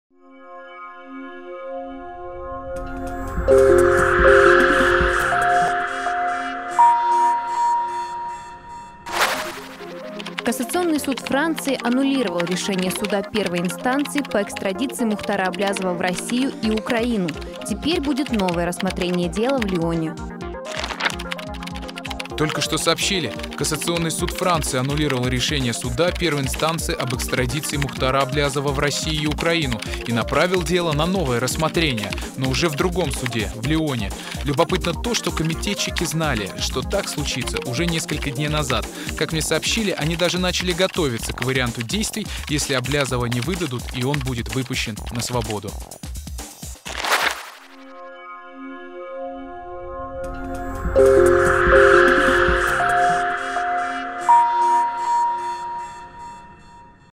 Суд Франции отменил экстрадицию Аблязова / 1612
Спустя несколько месяцев кассационный суд Франции все-таки аннулировал решение суда первой инстанции об экстрадиции экс-главы БТА-банка Мухтара Аблязова в Россию и Украину.